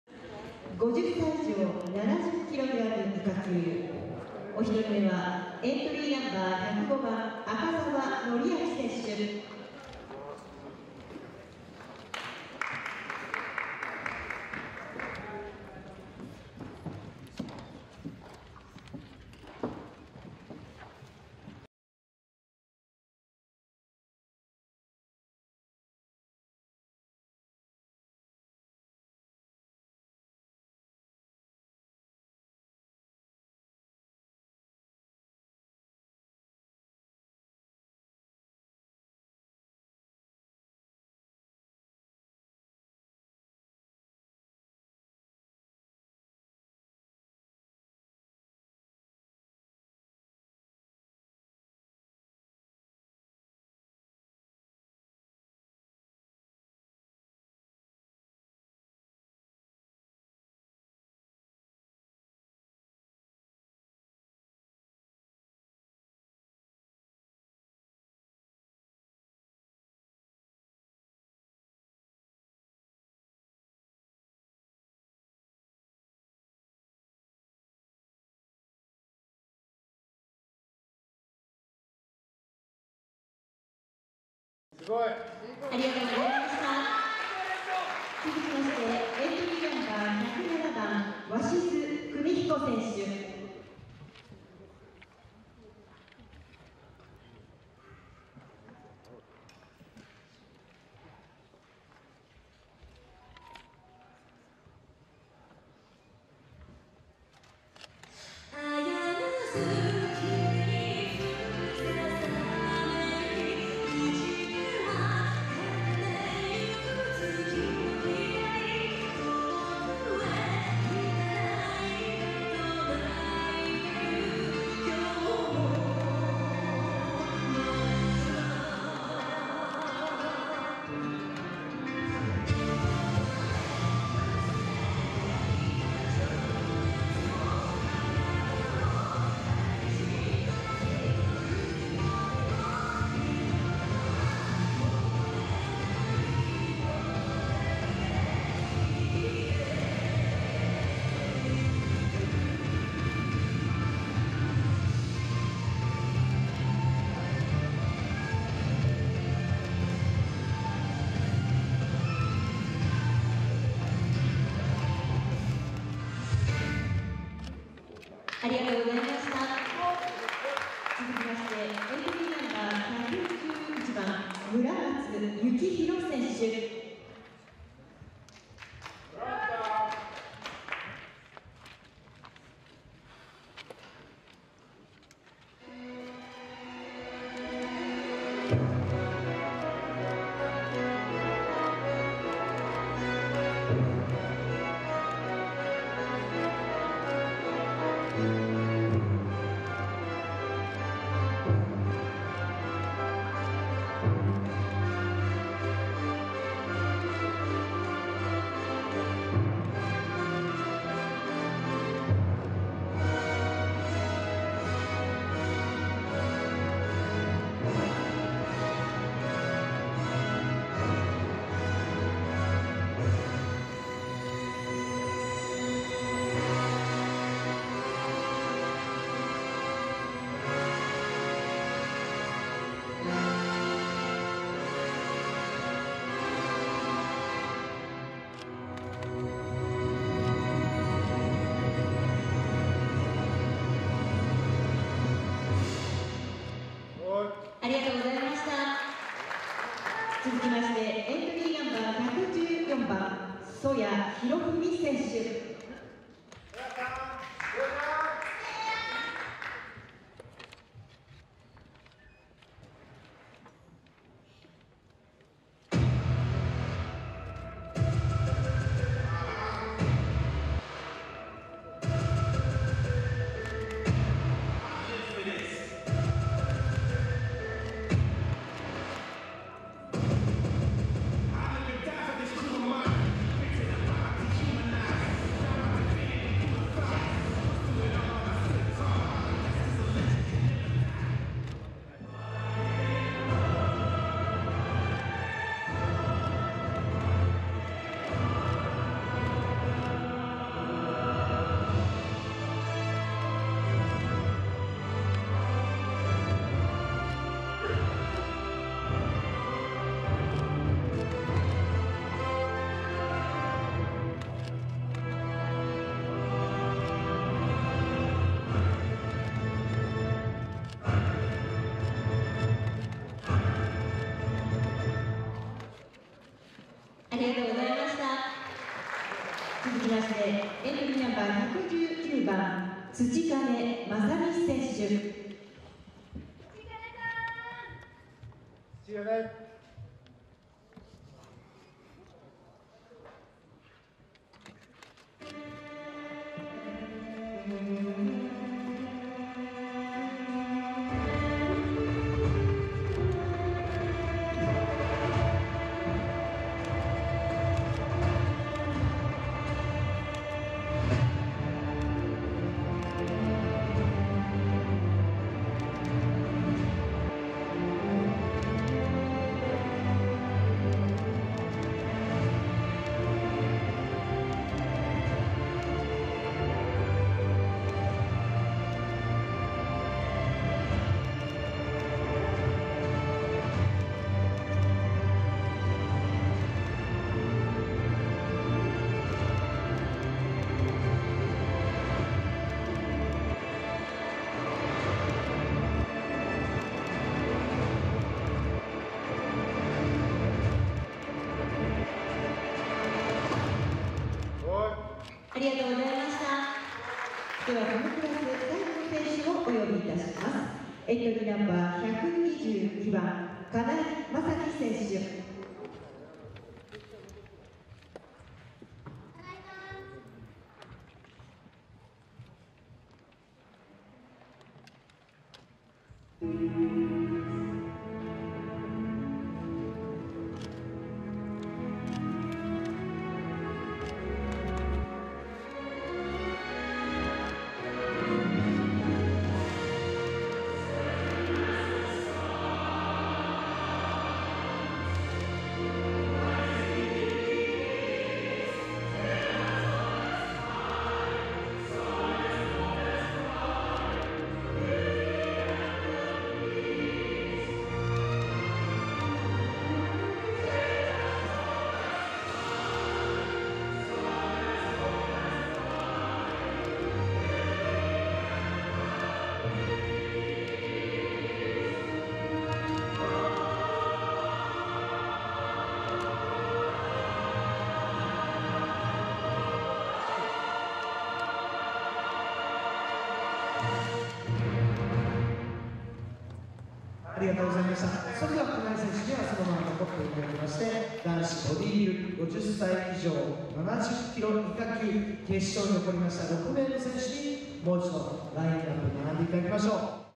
男子50歳以上70kg以下級フリーポーズ｜2023｜第35回日本マスターズ
8/27（日）に開催された『第35回日本マスターズ選手権』の男子50歳以上70kg以下級に出場した選手たちのフリーポーズ。

▼出場選手
0:00 赤澤範昭
1:35 鷲巣国彦
2:52 村松幸大
4:13 征矢洋文
5:35 土金正巳
6:50 金井雅樹

撮影／木村雄大

【WEB Magazine VITUP!】
トップページ